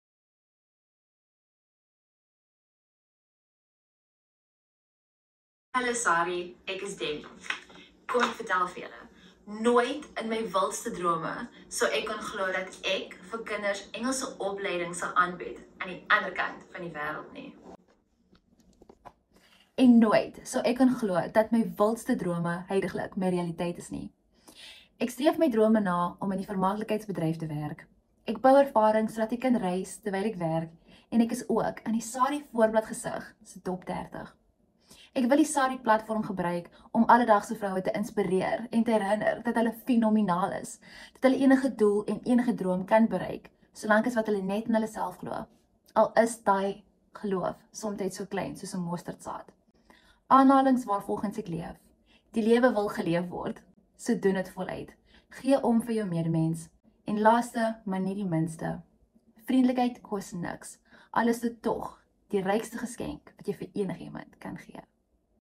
SARIEvoorbladgesig se Top-30: Demi Pedro
Ontmoet SARIEvoorbladgesig se Top-30: Demi Pedro werk vir haarself en is van Grabouw. Deel die opwinding met ons en hou SARIE se sosiale media dop, ons maak elke dag ’n finalis bekend.

In trotse samewerking met Isa Carstens Akademie en African Extracts Rooibos.